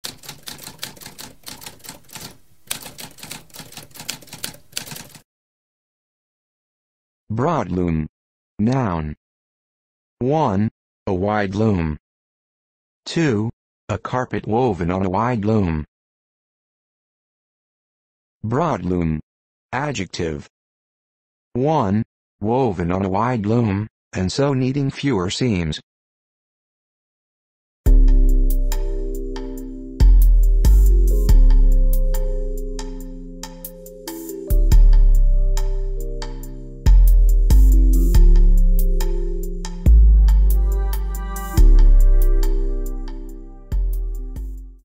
What does broadloom mean?
A spoken definition of broadloom.

Intro Sound:
Typewriter - Tamskp